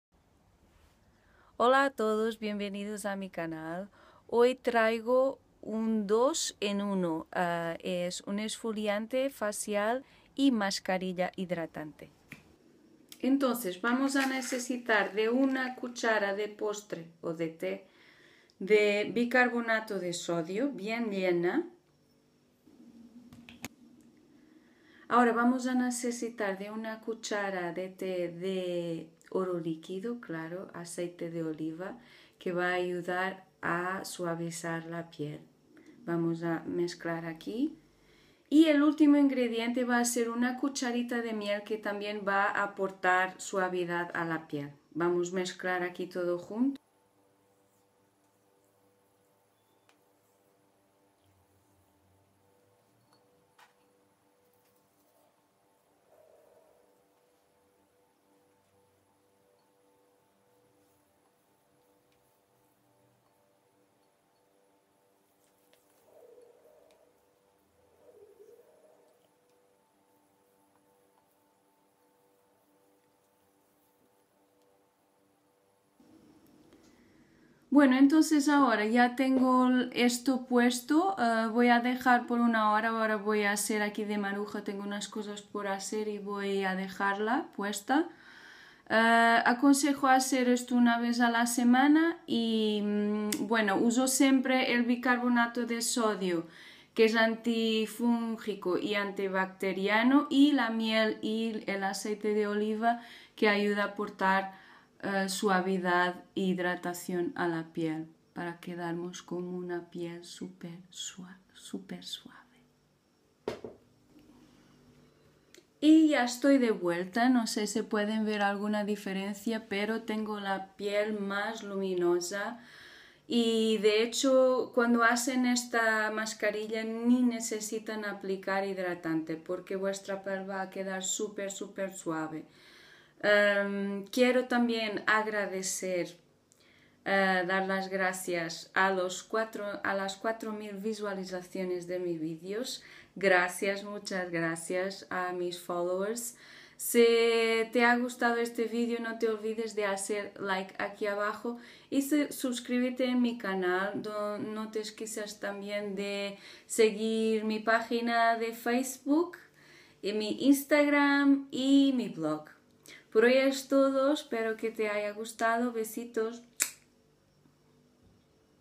🇪🇦DIY 2 en 1 exfoliante e mascarilla hidratante
1 cuchara tetera de bicarbonado de sodio
1 cuchara tetera de aceite de oliva
1 cuchara tetera de miel